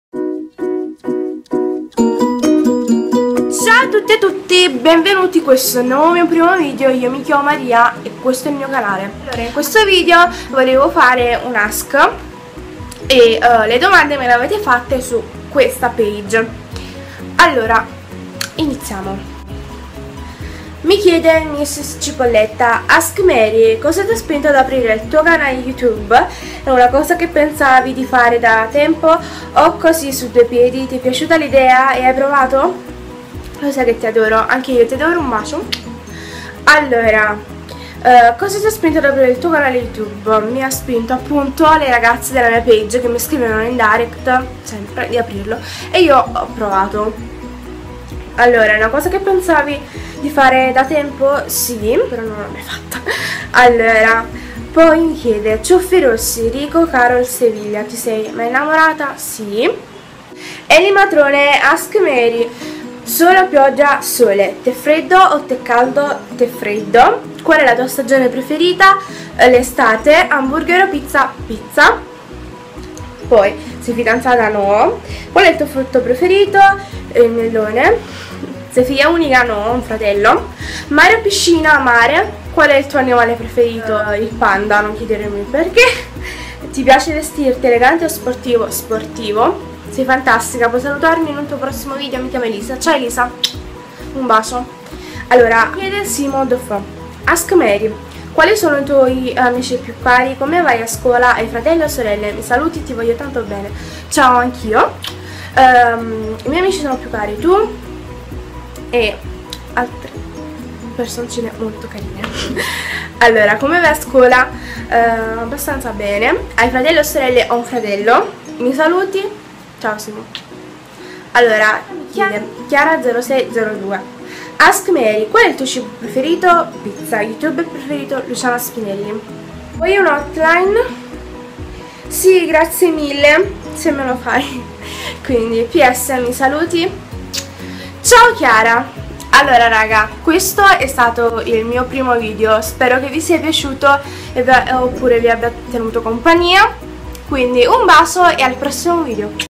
PRIMO VIDEO 💗
Benvenuti, nel mio primo video 💗

Su di me:
Mi chiamo Maria velotti,ho 13 anni
E quest'anno farò la 3ª media ,
Vivo  a Napoli
E amo ballare 💗